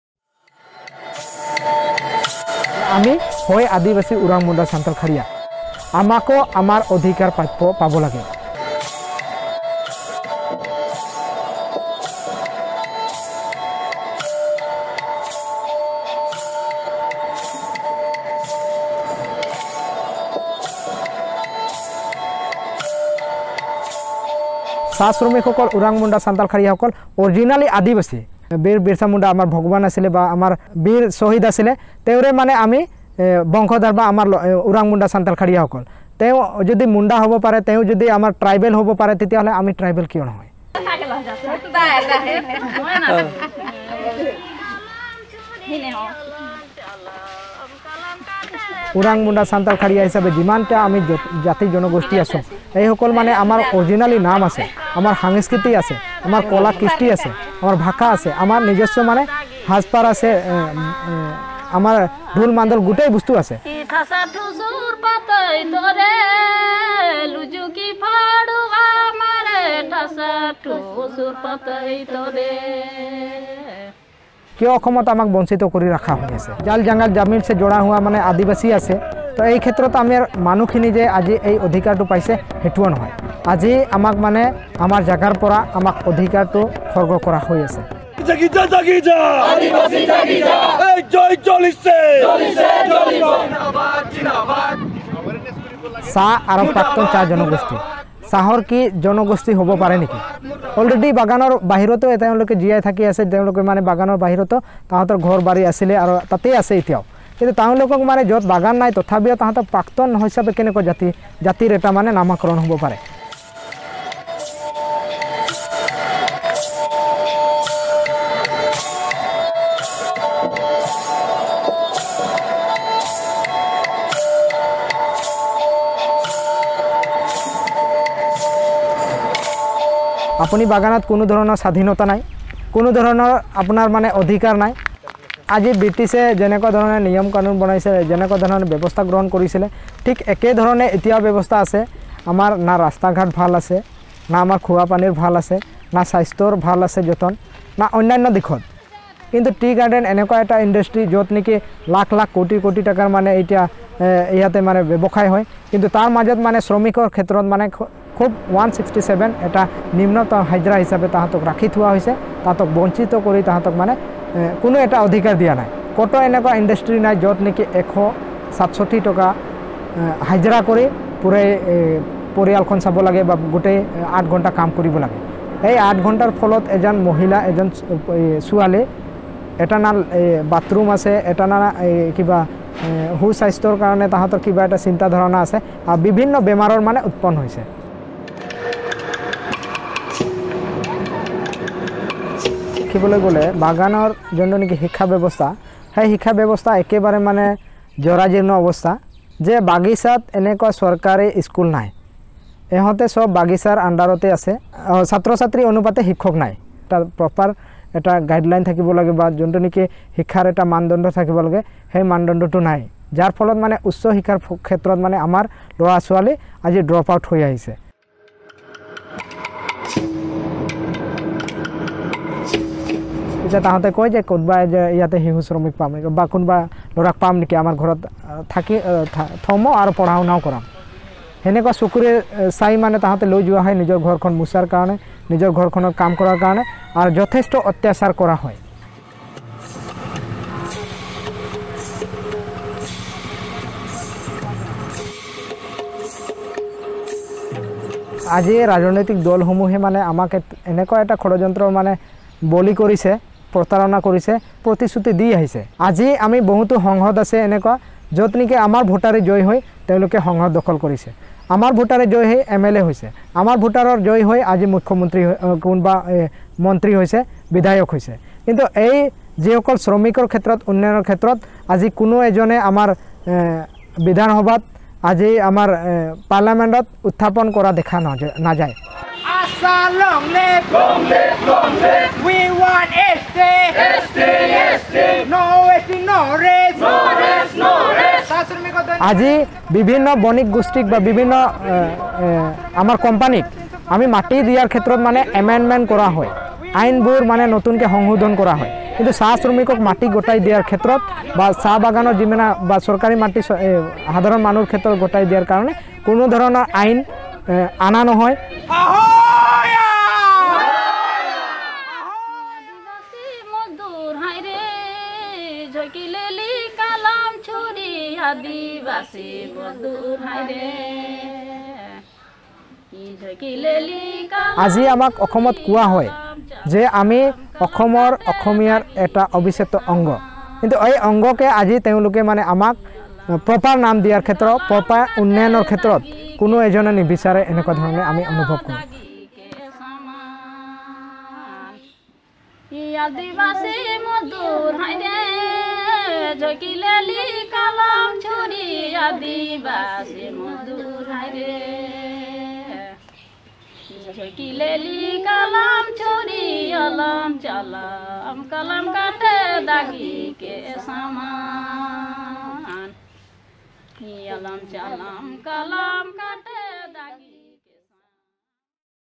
We Are Adivasis, Not Tea-Garden Tribes - Inside Assam's Tea-Gardens | Karwan e Mohabbat
"We, Orang, Munda, Santhal and Kharias are Adivasis. We demand our rights." Listen to the powerful voice of Mansukh Sankharika, public secretary of All Adivasi Student's Association of Assam (AASAA) as he walks us through the lives and history of the Adivasis residing in Assam's tea-gardens.

Officially referred to as 'tea-garden' tribes till date, Assam's tea-garden workers are asking for ST status. While ministers and elected officials refuse to listen to the demands of the Adivasis.